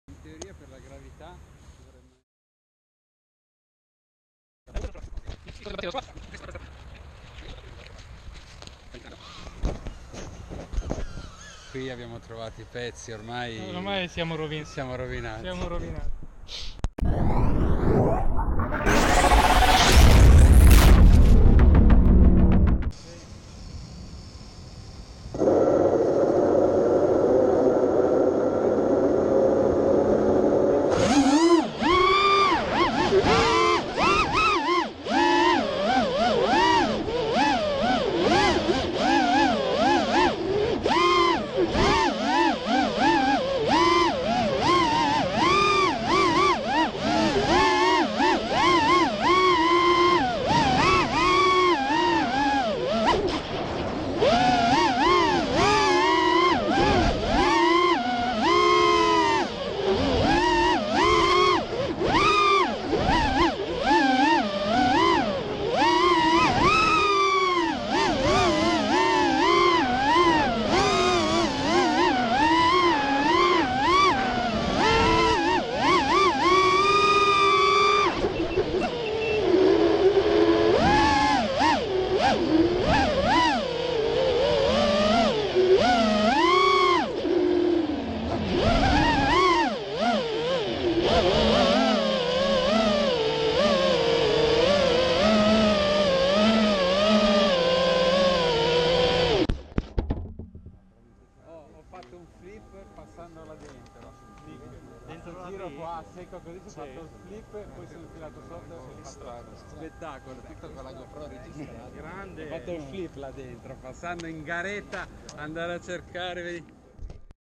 Chameleon and treetrack
Easy Day with some friends and my lovely Chameleon From Armattan
FC Revolt RF1 275
Oomph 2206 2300 Orange power
Vtx TBS unify HV